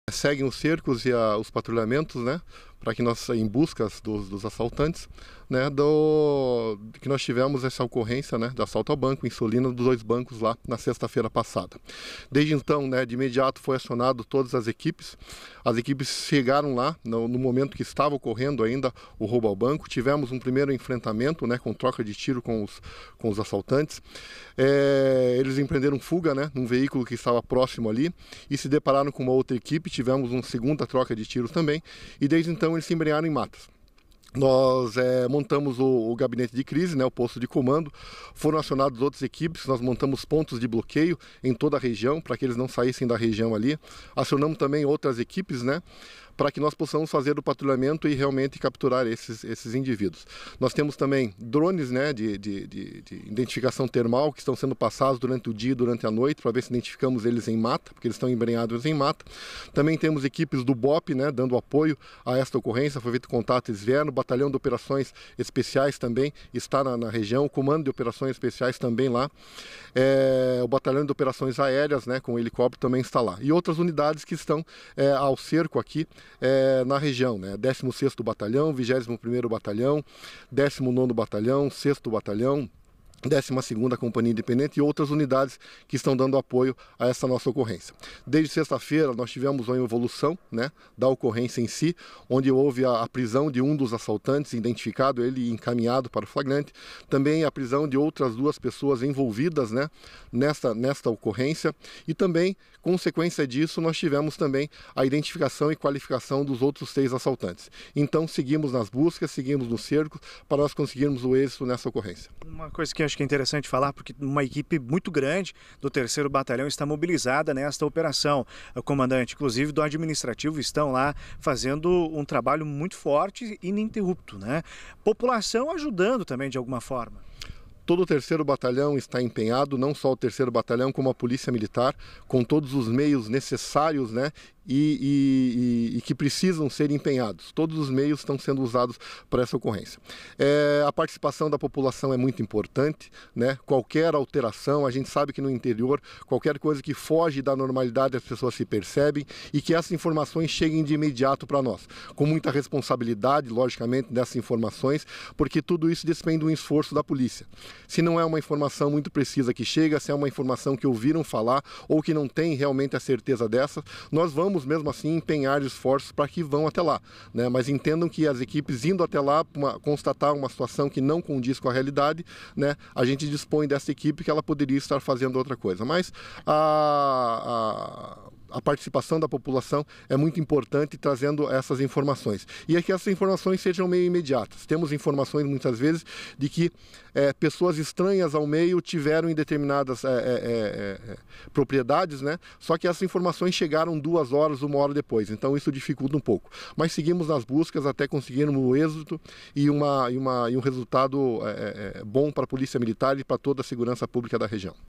Seguem as buscas dos homens envolvidos em assalto a agências bancárias
Segurança: Seguem as buscas a homens envolvidos em assalto a agências bancárias no município de Sulina. Nós conversamos hoje (09) pela manhã com o Tenente Coronel Heraldo, comandante do 3° BPM sobre o trabalho da polícia.